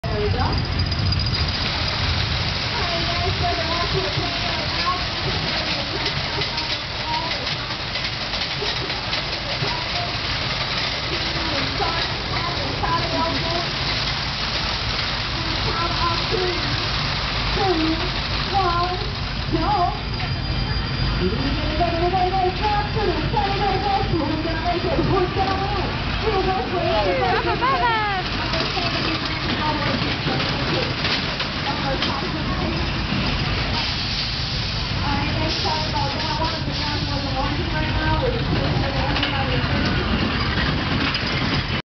Wacky Worm at Old Town Kissimmee 2009
Originally opening in 2008 and closing in 2014, the Wacky Worm (and Zamperla Windstorm next to it) occupied the land now used by Hurricane at Fun Spot America. This Wacky Worm now operates at Santa's Village AZoosment Park in Illinois.